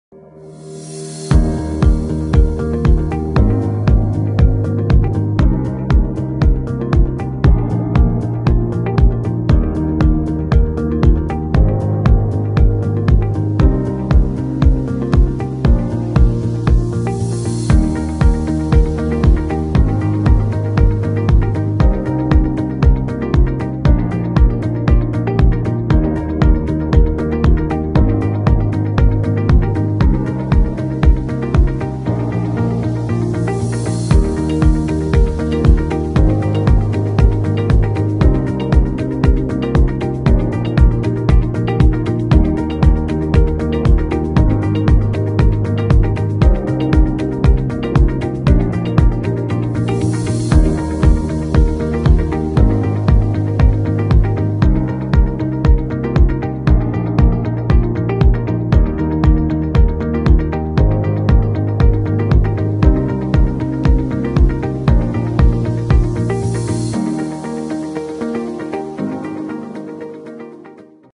Copala Los Cabos after Lidia
Thanks to the amazing hard work of our team this is how Copala Looks after the storm Lidia